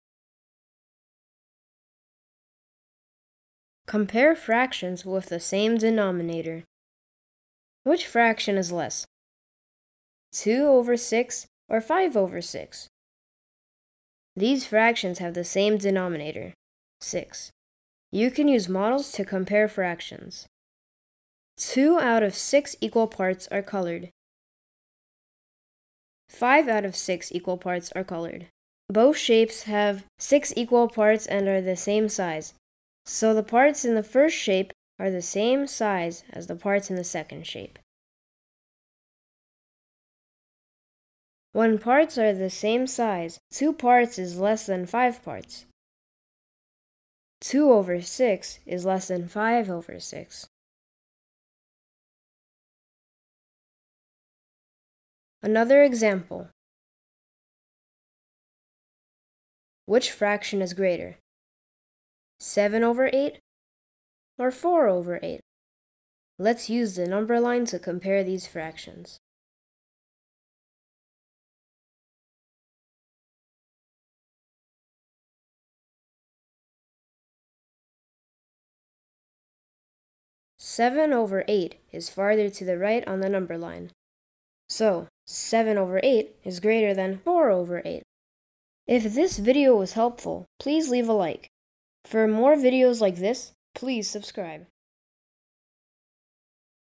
Compare Fractions with the Same Denominator. Grade 3
In this video you will learn how to compare fractions with the same denominator. Grade 3. Lesson 9.2.